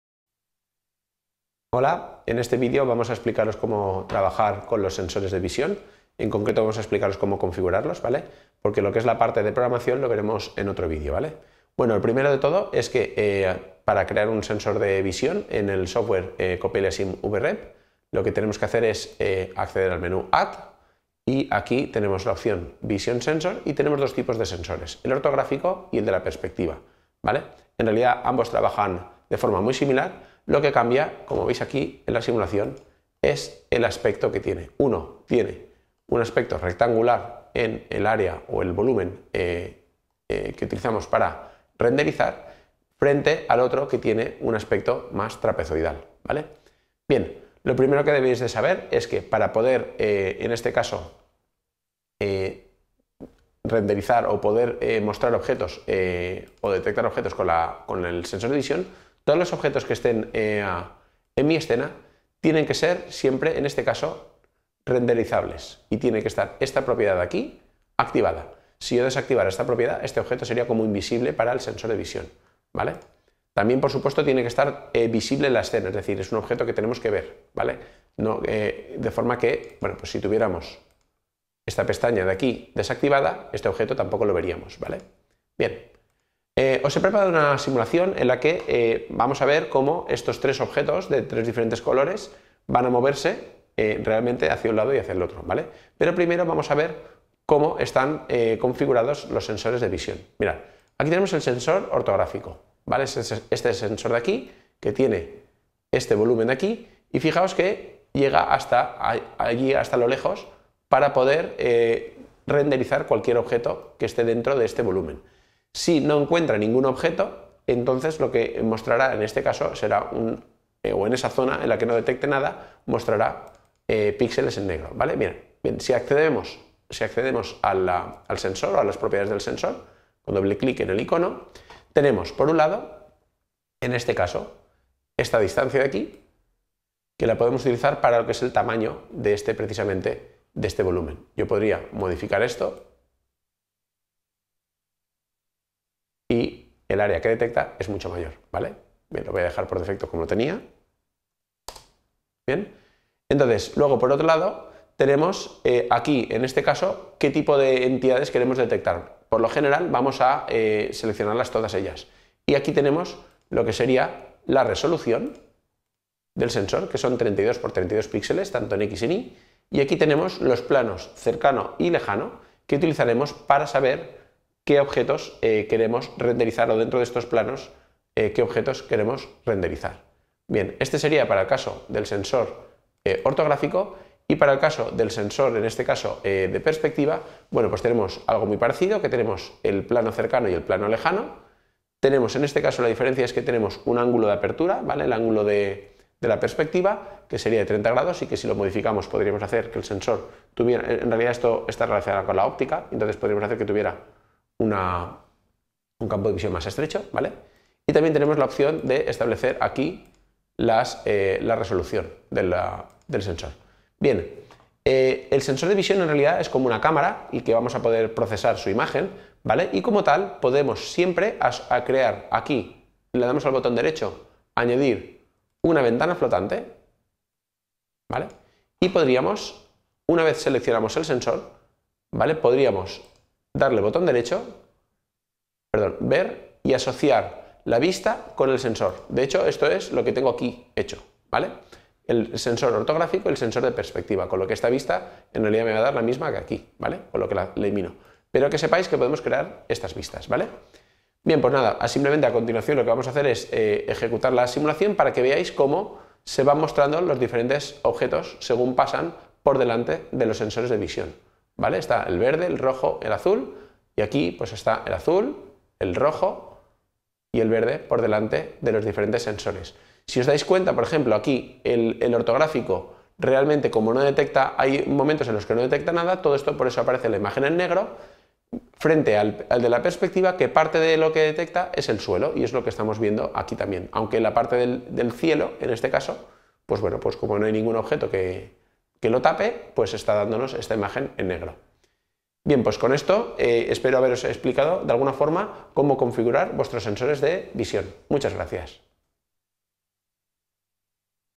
CoppeliaSim (V-REP): Sensores de visión| UPV
Título: CoppeliaSim (V-REP): Sensores de visión

Descripción: En este vídeo se explica el uso de sensores de visión en el software CoppeliaSim. Armesto Ángel, L. (2020). CoppeliaSim (V-REP): Sensores de visión.

Descripción automática: En este video, se enseña cómo configurar sensores de visión en el software CoppeliaSim (V-REP), abordando sus dos tipos: ortográfico y de perspectiva. Se destaca la importancia de activar la opción de renderizado para detectar objetos con los sensores de visión, que también deben estar visibles en la escena.

Se muestra una simulación con tres objetos de diferentes colores moviéndose, mientras se explican las características de configuración de los sensores, como el tamaño del volumen de detección, el tipo de entidades detectables, la resolución y los planos cercano y lejano. Para el sensor de perspectiva, se toca el tema del ángulo de apertura y su influencia en el campo de visión.

Además, se explica la posibilidad de procesar la imagen capturada por los sensores, similar a una cámara, incluyendo la creación de ventanas flotantes asociadas a la perspectiva del sensor. Finalmente, se realiza una demostración práctica de la simulación, mostrando cómo los objetos pasan por delante de los sensores y se reflejan diferencias en sus vistas, por ejemplo, las áreas no detectadas aparecen en negro. Con esto, se finaliza la orientación sobre la configuración de sensores de visión.

Autor/a: Armesto Ángel Leopoldo